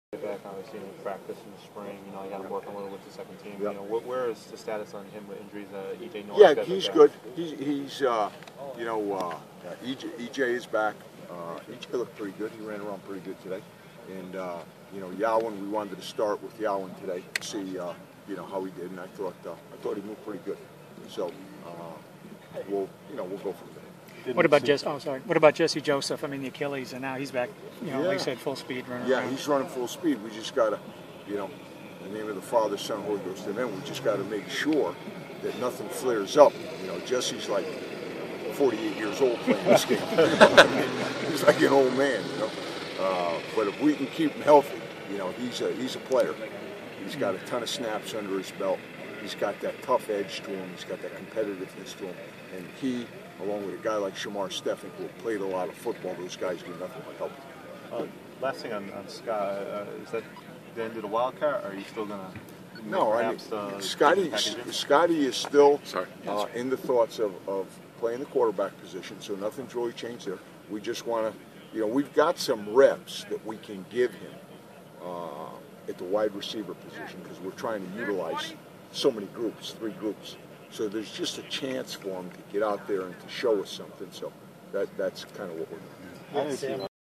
UConn football coach Paul Pasqualoni after first day of fall camp 2013
UConn head coach Paul Pasqualoni takes a look at the first day of fall camp 2013.